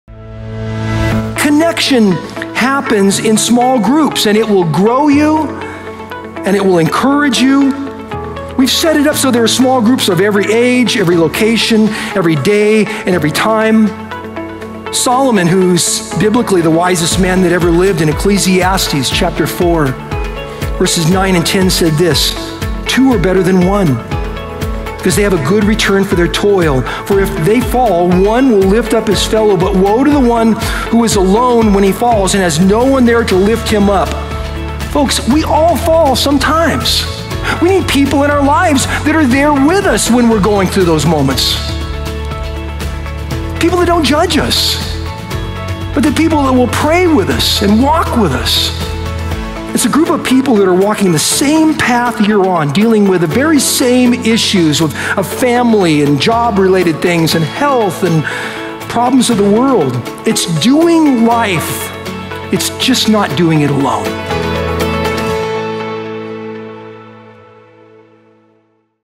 Connect to Grow - Sermon Short
Being a part of a church family means having a group of people around you to support you, pray for you, and help you grow in your faith. When difficult times come, a small group of fellow believers is there to help you through, and it's how God provides comfort for those facing hardships. Don't miss the opportunity to connect with others and grow your faith together - find out more about small groups at Highlands at HighlandsChurch.org/Groups